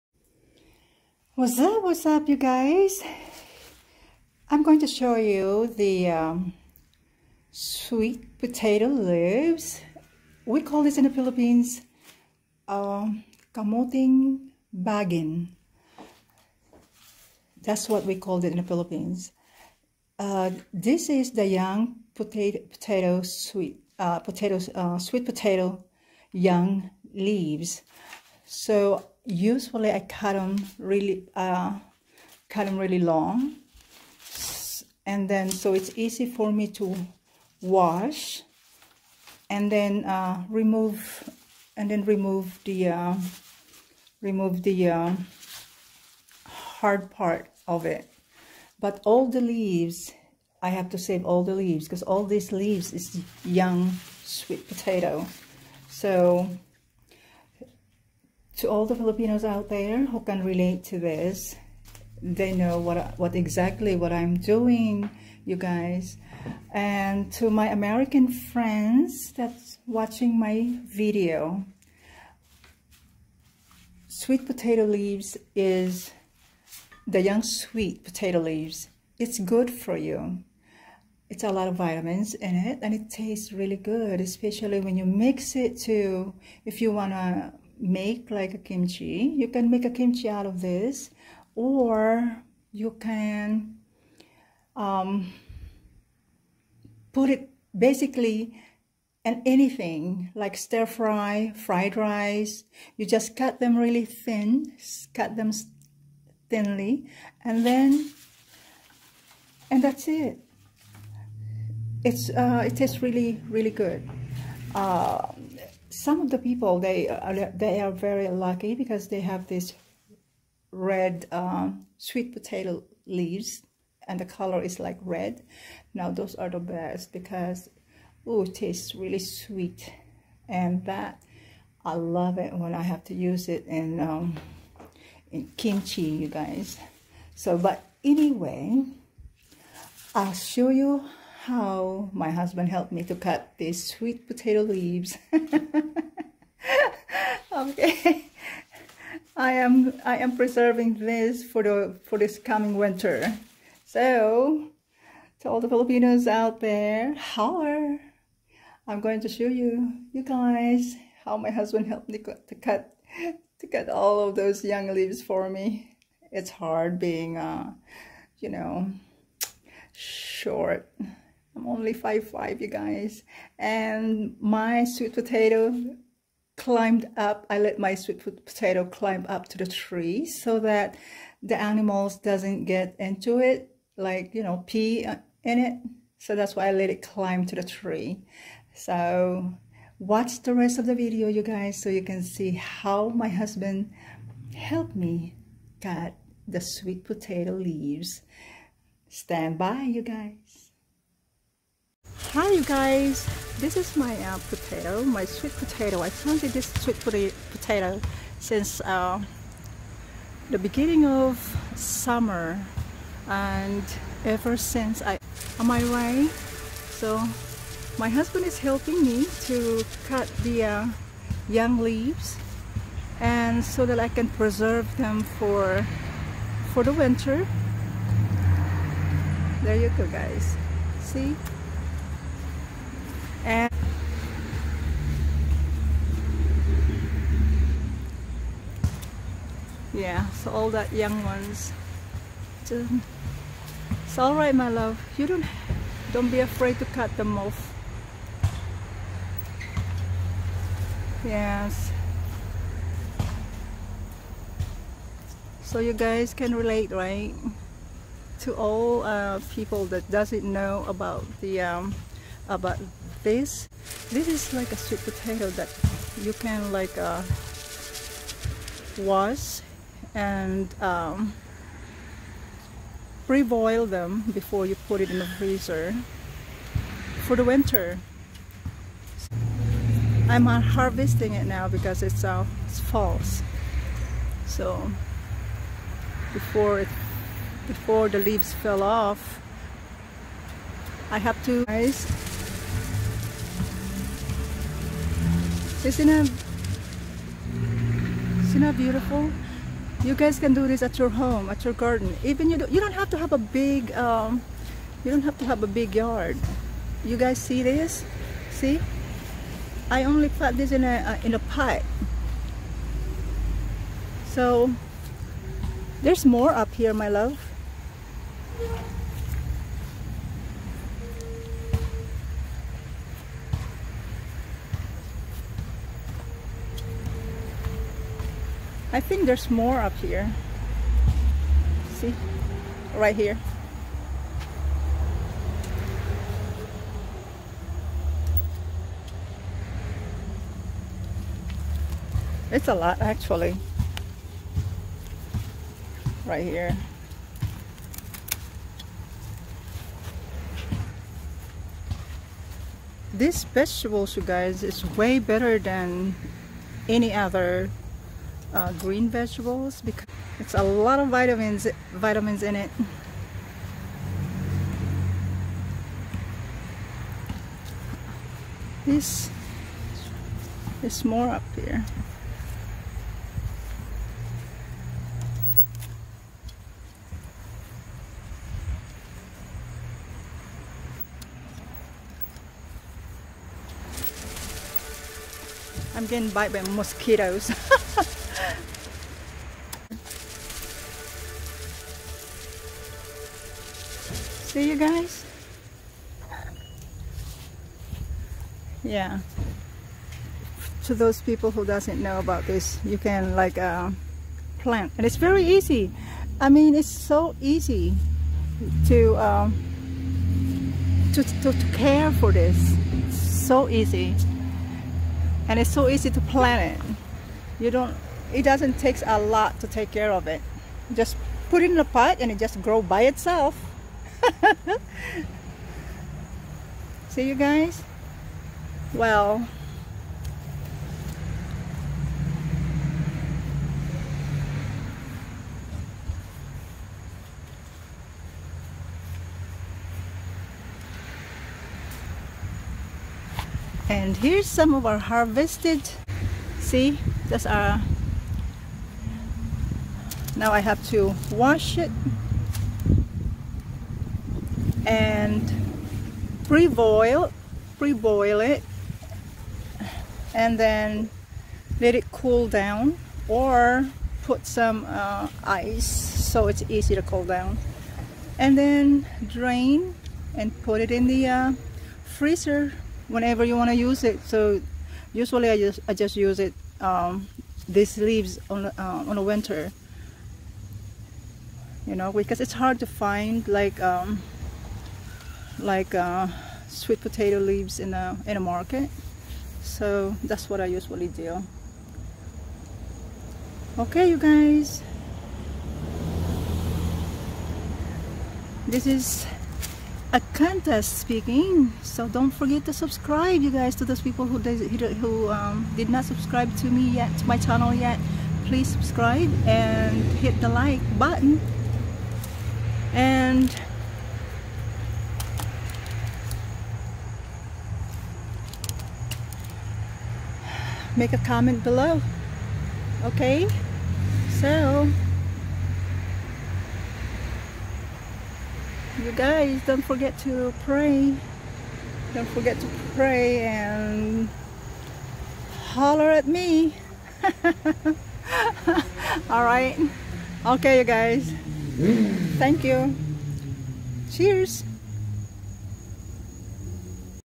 How to grow sweet potatoes in a pot 🙏
This is a video showing how you can save money by growing sweet potatoes in a pot for a small backyard